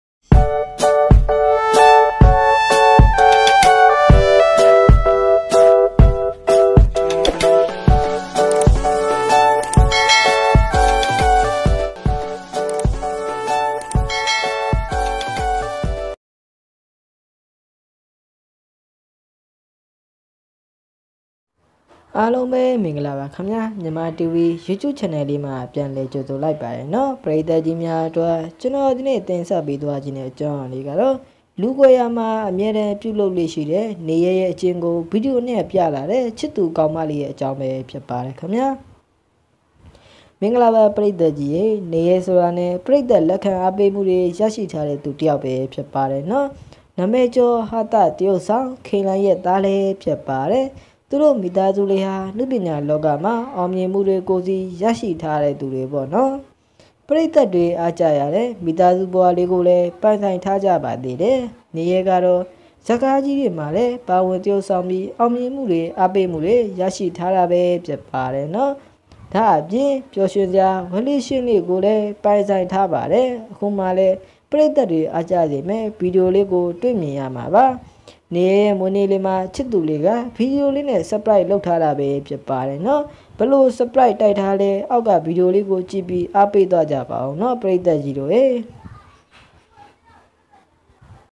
လူကွယ်ရာမှာအမြဲတမ်း ပြုလုပ်လေ့ရှိတဲ့နေရဲရဲ့အကျင့်ကိုvideo နဲ့ပြလာတဲ့ချစ်သူကောင်မလေးmyanmarcelenews
လူကွယ်ရာမှာ သူ့မကို အမြဲတမ်း ပြုလုပ်လေ့ရှိတဲ့ နေရဲရဲ့ အကျင့်ကို video နဲ့ ပြလာတဲ့ ချစ်သူကောင်မလေးmyanmarcelenews